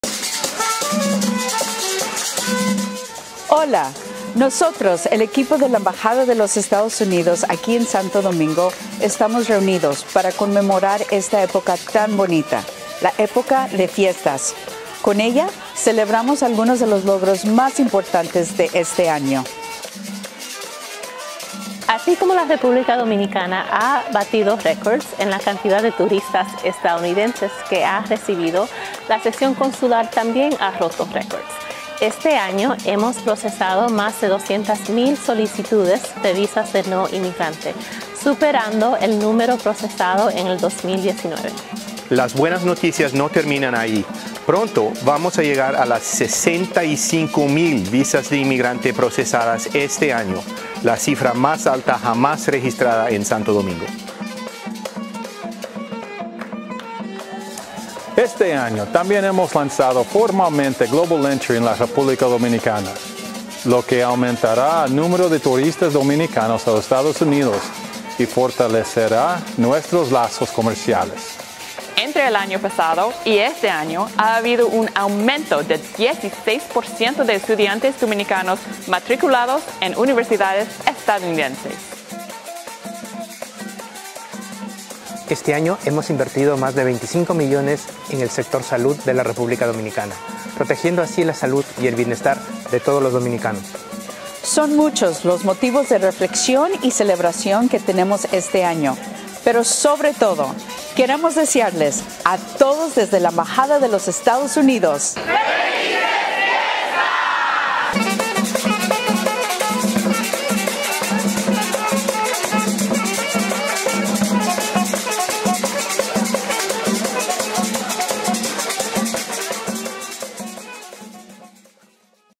¡Felices Fiestas 2023!
🎅🏽🎄Nos llena de alegría celebrar esta hermosa época con ustedes, y al contagioso ritmo de su música 🎷. En estos días, es inevitable reflexionar sobre lo que hemos logrado juntos, y aquí le mencionamos algunos de los éxitos de este año. Gracias por hacerlo posible y porque hemos podido seguir reforzando los lazos de amistad que unen nuestros dos países. ¡𝗙𝗲𝗹𝗶𝘇 𝗡𝗮𝘃𝗶𝗱𝗮𝗱 𝗮 𝘁𝗼𝗱𝗼𝘀!